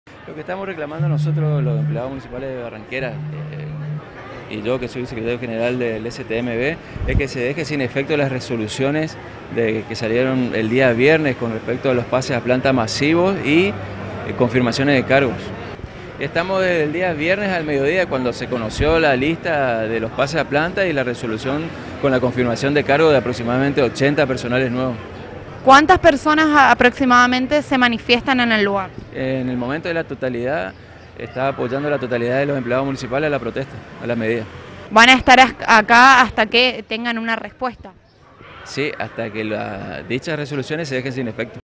C9 - BARRANQUERAS; “OPERATIVO RETIRADA”
TRABAJADORES MUNICIPALES RECLAMAN PASES A PLANTA INJUSTOS
En el marco de la finalización del cargo de intendente de Alicia Azula en diciembre debido a que Magda Ayala fue electa intendente en las elecciones del corriente mes; Azula indicó 80 pases a planta  por lo que trabajadores del municipio se manifiestan desde el día viernes y piden que se dé de baja el decreto ya que según los trabajadores “son pases injustos para las personas que venimos a trabajar todos los días”
DANIEL GONZALEZ - TRABAJADOR